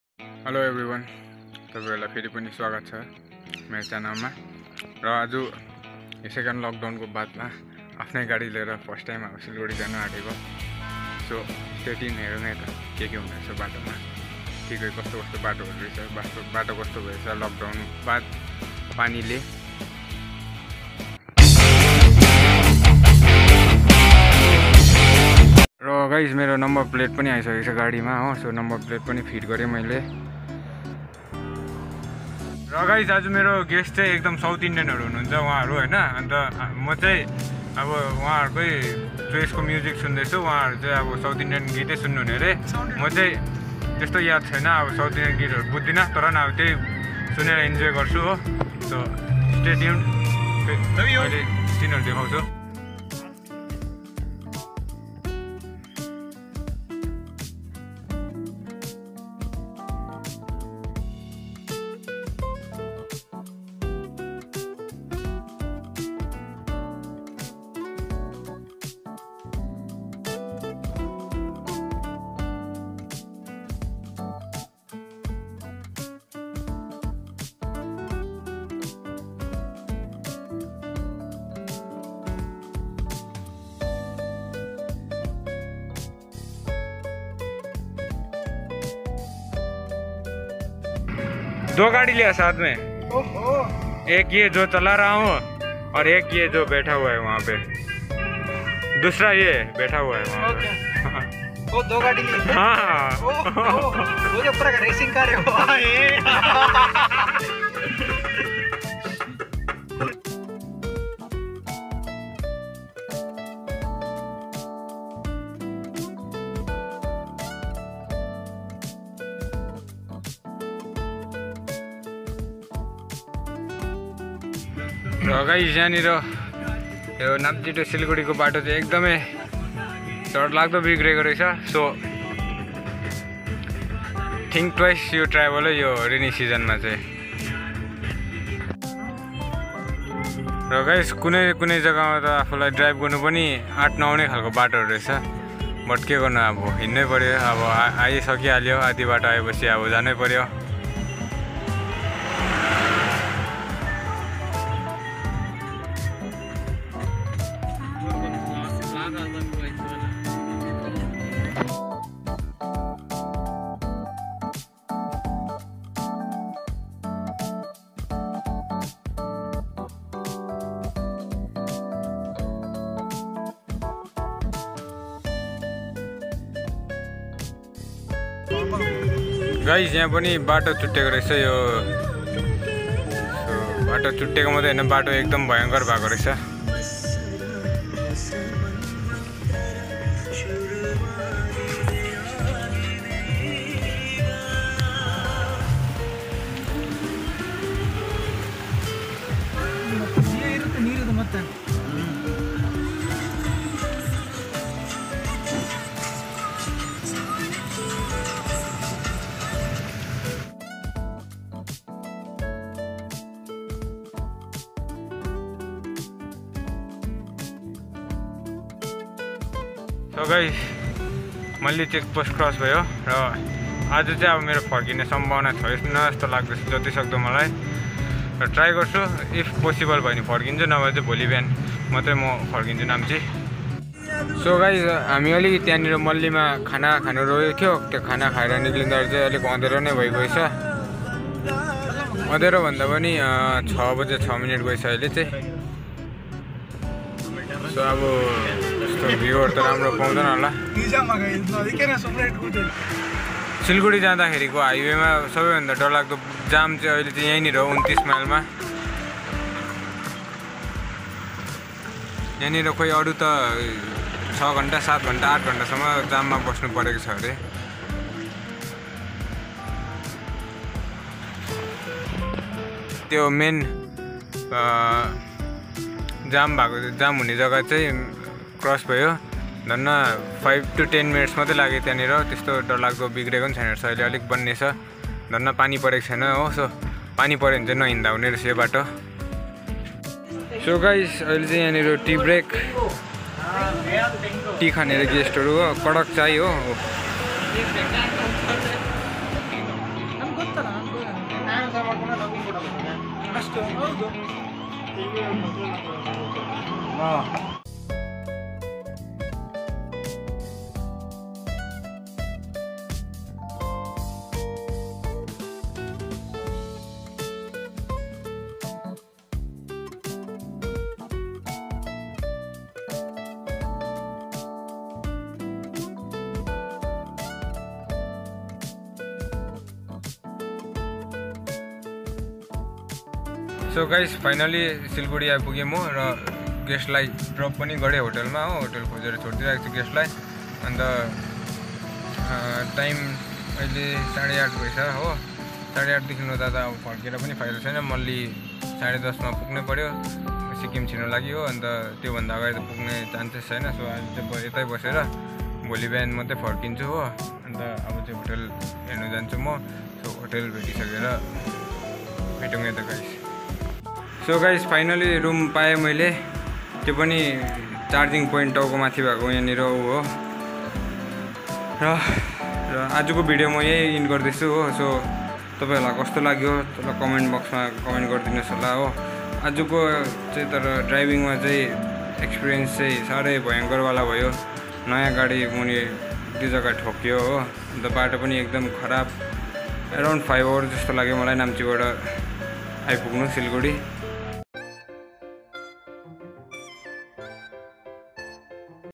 Namchi to siliguri drive|| insane memer|| WagonR rainy season drive|| Nepali vlog
drive  From Namchi to siliguri at rainy season 🥵,